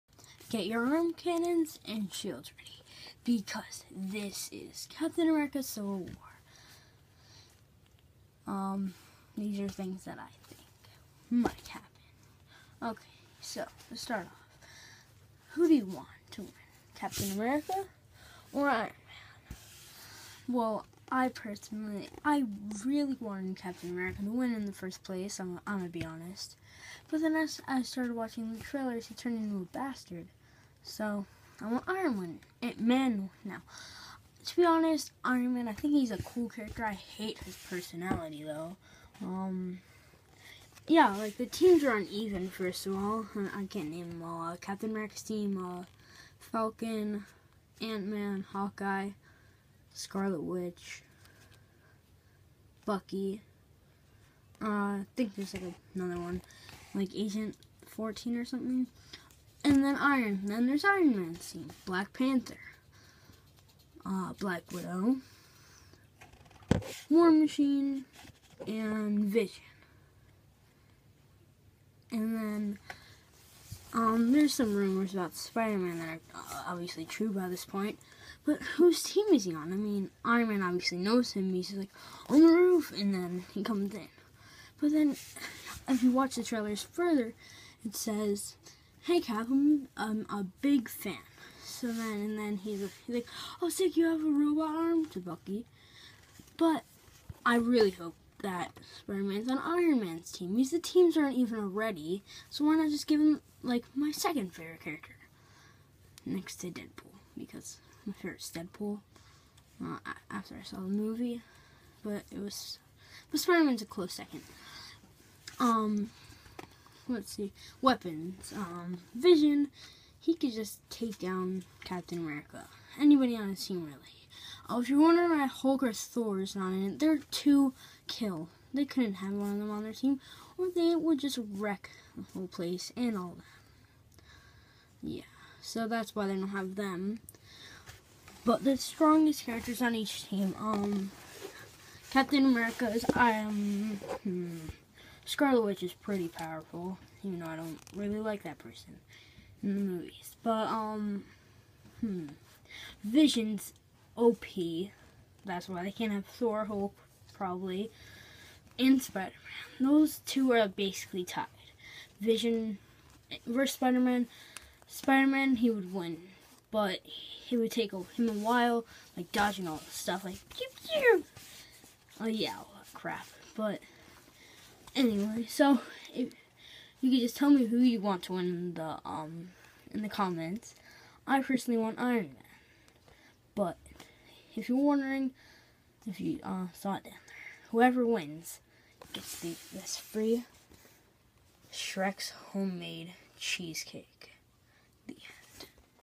CAPTAIN AMERICA CIVIL WAR THOUGHTS
Star fox talks about what he wants to happen in Captain America civil war